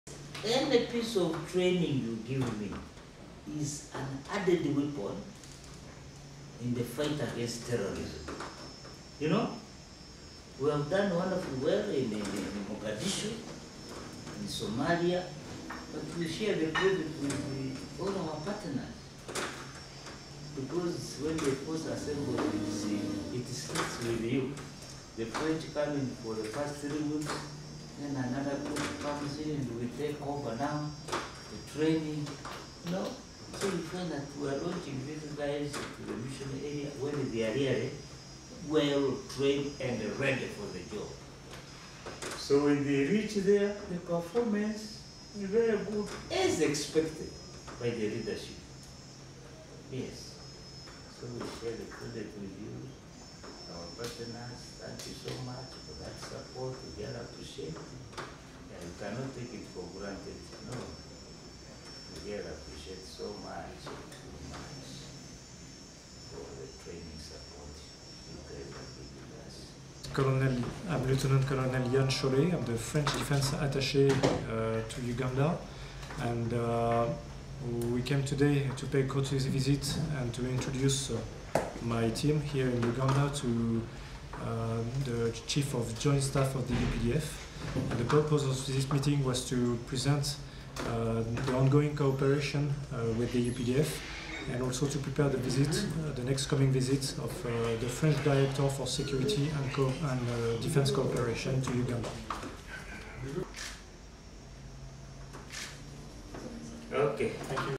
Maj Gen Bakasumba Hails French Training Support To UPDF
The Chief of Joint Staff, Maj Gen Jack Agonza Bakasumba, has commended the French government for its training support to the Uganda Peoples’ Defence Forces (UPDF), which has significantly enhanced the fight against terrorism.
Maj Gen Bakasumba made these remarks as he hosted the French Defence Attaché, Lt Col Yam Chollet, who paid a courtesy visit to him at the Ministry of Defence and Veteran Affairs (MODVA) Headquarters in Mbuya.